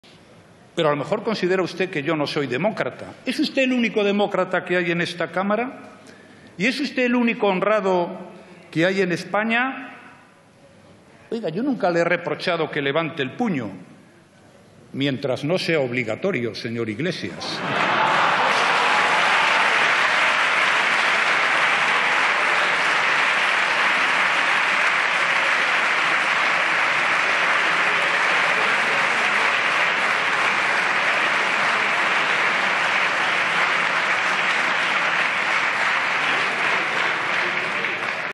RAJOY zasca a PABLO IGLESIAS
Mariano Rajoy no pudo evitar hacer algunas bromas tras la intervención de Pablo Iglesias durante su respuesta en el Debate de Investidura.